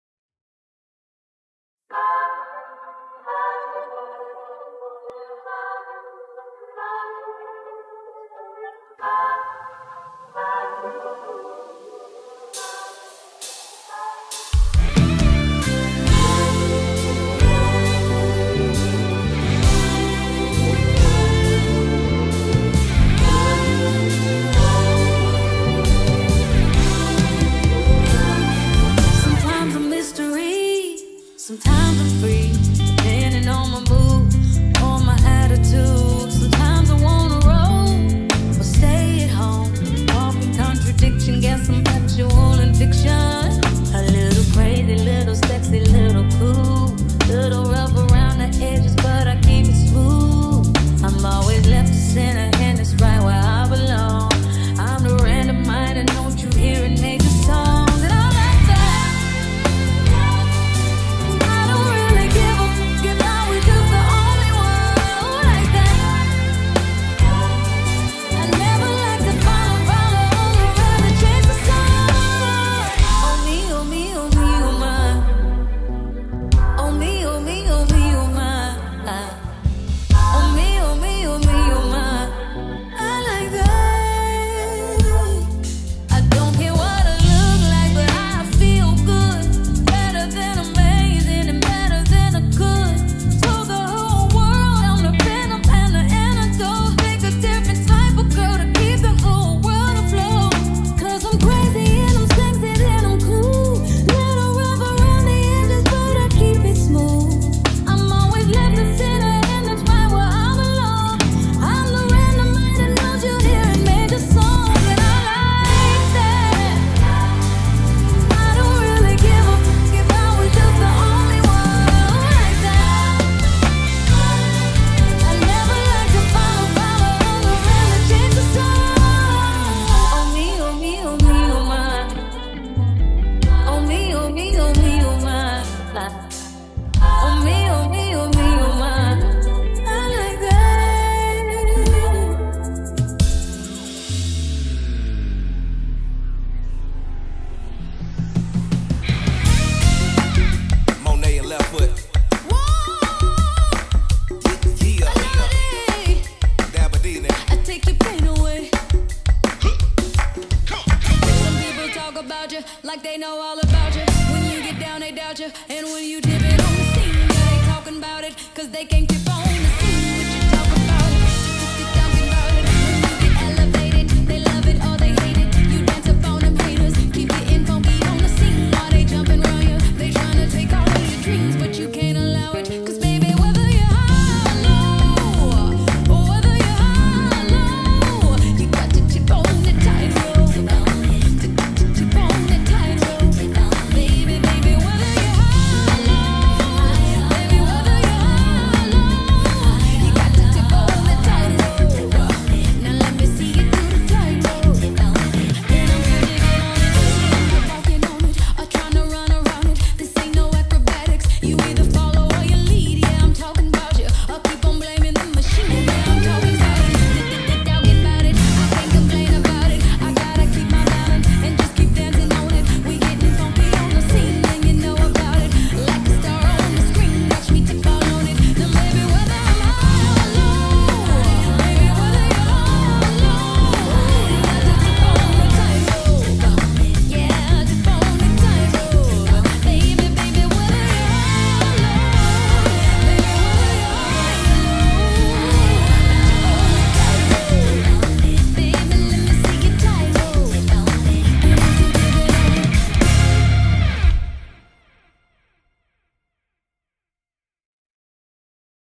Janelle Monae | LIVE ARRANGEMENT
Hey everybody!
Back with another arrangement. This time, we're highlighting one of the most unique artists of our generation, Janelle Monae. This arrangement was started just before the quarantine started and the drums were recorded during at the beginning of the quarantine (hence the quarantine beard and the desperate need for a haircut lol) but i'm glad that I could get this out to you guys.

Thanks to my guys who helped me bring this arrangement to life. Be sure to follow them for all the great stuff that they do!
Kendrick Fielder (@kendrickfielder) - Guitar
JScott Martin (@theglove0297) - Bass
yours truly (@tatum.wav) - Arrangement, Programming, Drums
Devin Smith (@ds16music) - Mix Engineer .